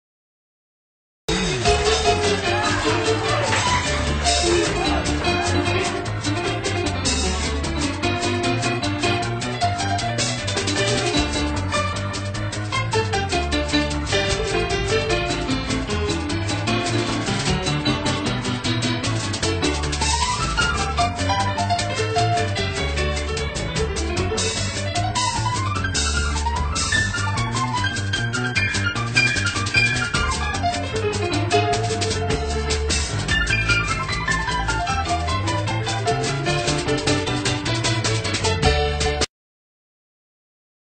Jackie Coogan "Shimmyin' in1920 @ Chaplin's studio..
Jackie Coogan ...'THE KID"...  Upon completion of filming 'The Kid,' Sir Charlie Chaplin invited the heads of First National (distributors of his films), to the post party.. This is a glimpse of said day..